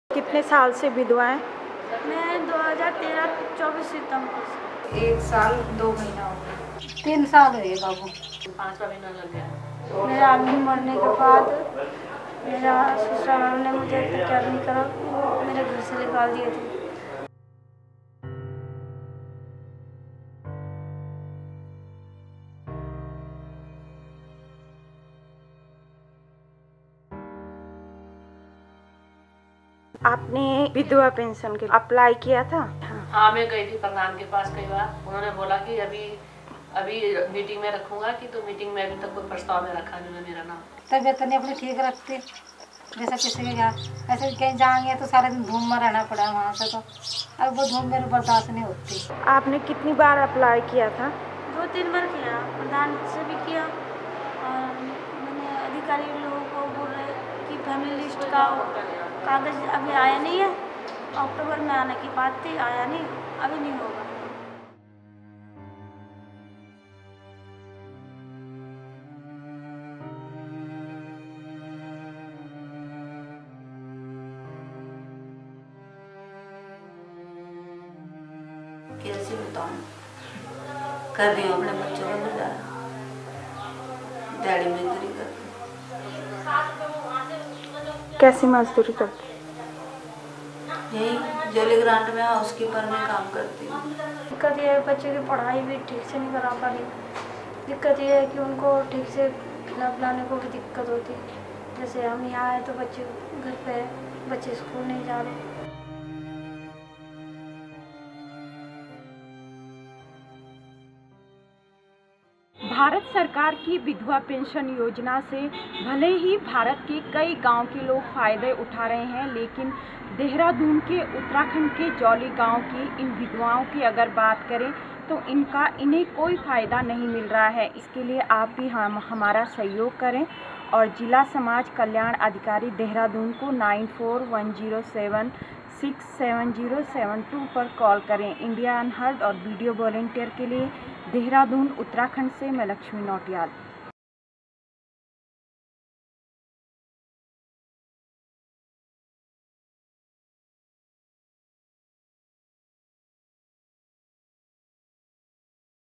4 Widows hoping for a pension in Uttarakhand | Luxmi reports for IndiaUnheard
Women in most parts of India face discrimination and exclusion due to the patriarchal social setting in all spheres of their lives. Widows of India, especially in rural India, face more discrimination and exclusion because with the demise of their husband because their social standing and identity are also considered to have ‘died’. Pooja was ousted from her in-law’s house in Jolly village of Dehradun, along with her two children, after her husband died. ” After my in-laws threw me out, my maternal home also didn’t take me back. I had no place to turn to with my two children in tow,” Pooja tells Luxmi Nautiyal, Video Volunteers Community Correspondent from Dehradun, Uttrakhand. Belonging to a Below Poverty Line, it is particularly hard for Pooja to sustain her family and education of her children and finding employment was especially tough since Pooja is illiterate.

Like Pooja, most women in rural India are economically dependent on husbands due to little or not education (education percent) ; they face a lot of adversities after their husband’s demise. To help widows be financially less dependent on others, the Government of India offers a pension plan ‘Vidhwa Pension Yojana’. Under this scheme registered widows belonging to the Below Poverty Line (BPL) households get government support of Rs. 300 per month. While this amount is insufficient to sustain a family comfortably, it has value for a household that fights for survival every day.

Pooja had applied for the widow pension several times since the demise of her husband, but her appeals have met neglect from authorities at Jolly village. “I have given at least two applications to the village head and even requested the authorities to look into the matter,” but the authorities have so far not taken any definitive action to help Pooja.

Pooja works at less than Rs. 3,000 a month, as a help in the local hospital of the village of Jolly. “The wages are just about enough to bring food home but my children can’t go to school because it is difficult to make ends meet,” tells a visibly tired Pooja.

Many Poojas in Jolly need your help

The struggle is not Pooja’s alone. The village has several other widows who have been waiting for their widow pension since a few years. Lata makes ends meet as a housemaid and Sureshna, depends on farm labour to sustain her family. They all have made several applications and appeals to the village head and the local social welfare department.

While the government has installed schemes for the benefit of BPL families, the lax attitude of administrators at the ground level has been a hindrance in effective implementation. These women deserve to live their lives with dignity and get the pension, that is their right.

Call the Social Welfare Officer of Dehradun, Uttrakhand on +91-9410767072 and demand that these women of Jolly get their widow pension at the earliest
Community correspondent Luxmi reports from Uttarakhand for Video Volunteers.